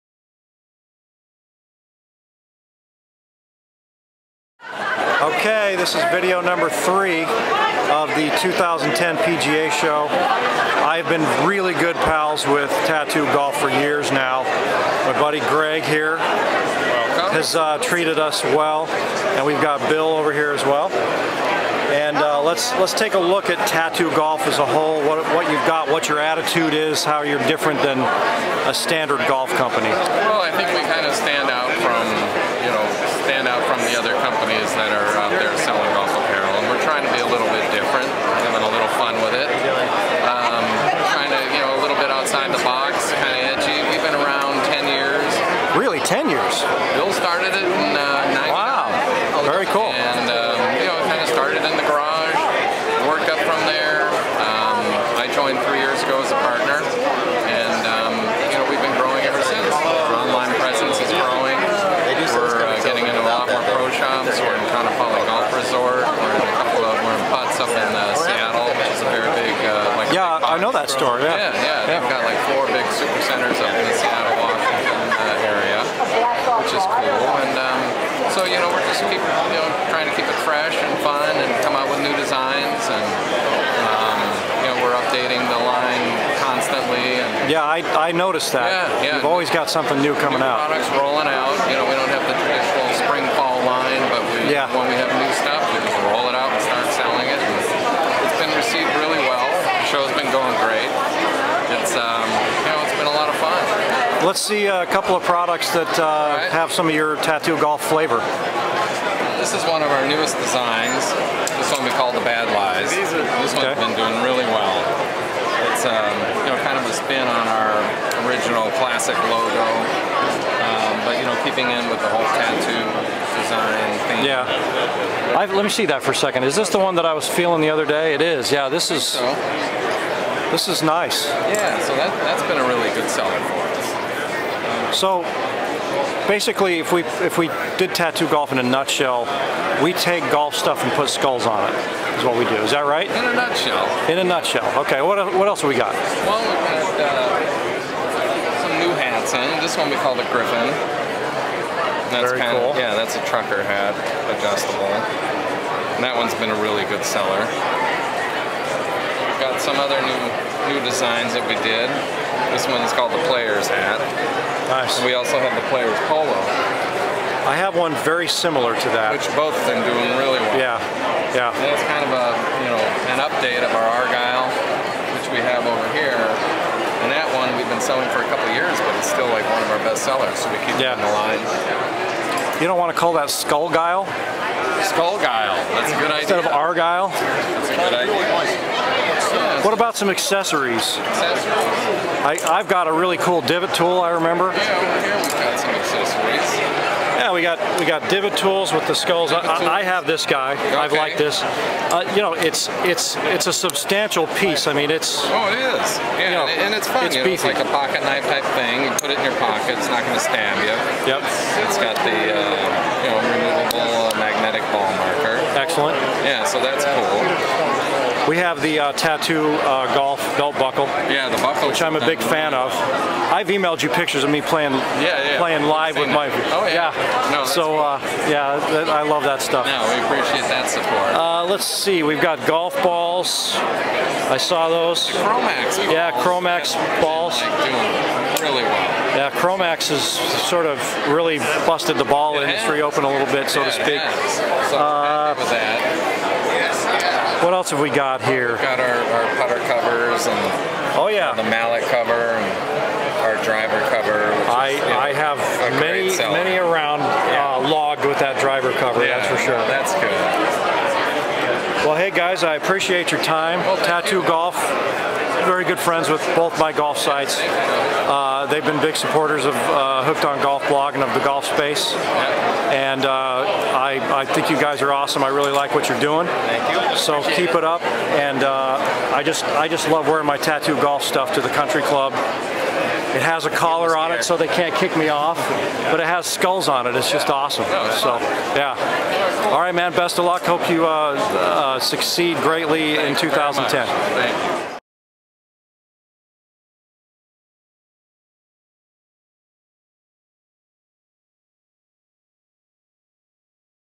Tattoo Golf 2010 PGA Merchandise Show
I interview my good friends from Tattoo Golf at the 2010 PGA Merchandise Show